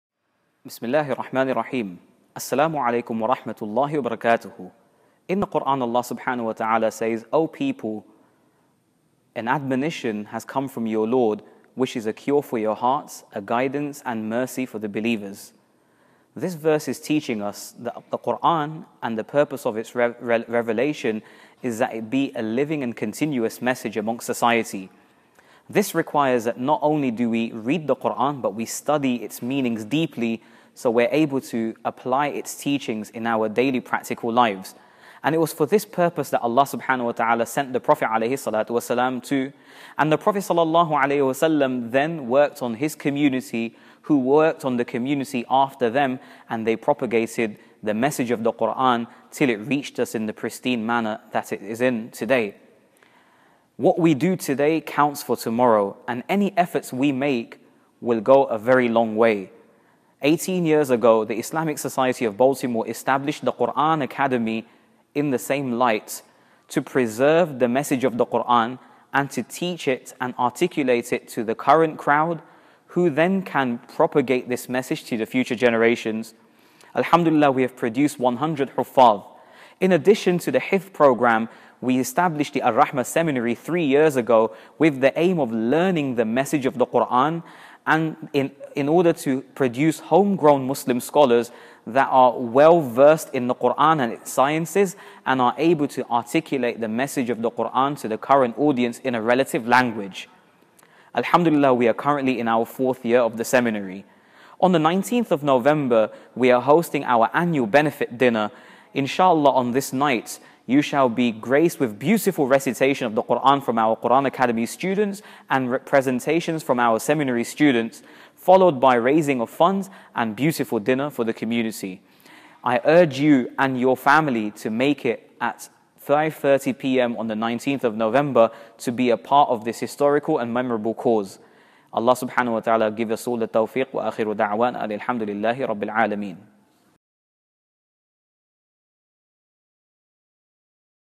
Night of Divine Inspiration 2016 - Shaykh Yaseen
Listen to a message from Shaykh Yaseen on the important work being done by the Quran Academy's programs (Hifdh and Seminary) and understand why we need your presence and support on November 19th at 5:30 PM at this annual benefit dinner!